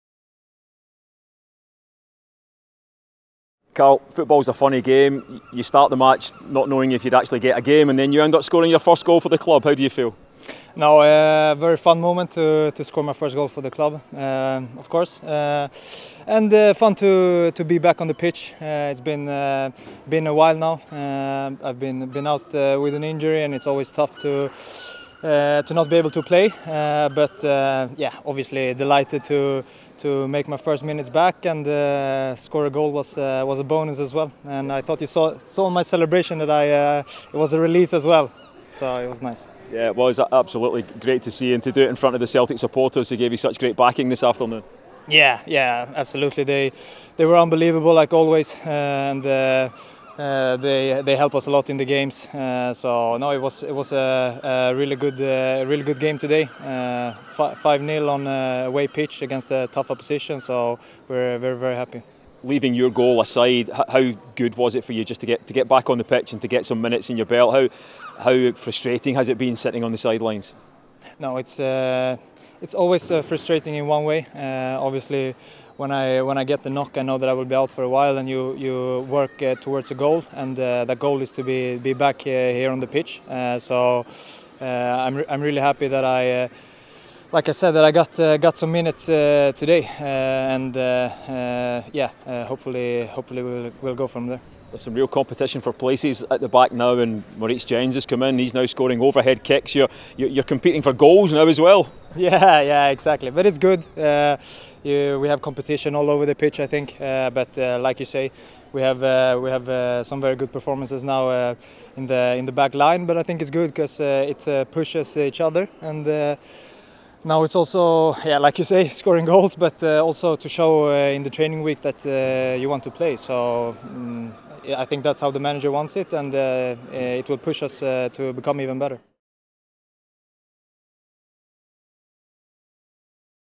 Carl Starfelt On The Match | Kilmarnock 0-5 Celtic | Five-Star Celts!
🎙 Carl Starfelt speaking on the match after scoring his first CelticFC goal.

"I was delighted to get my first minutes back and to get a goal was a bonus as well!" 💬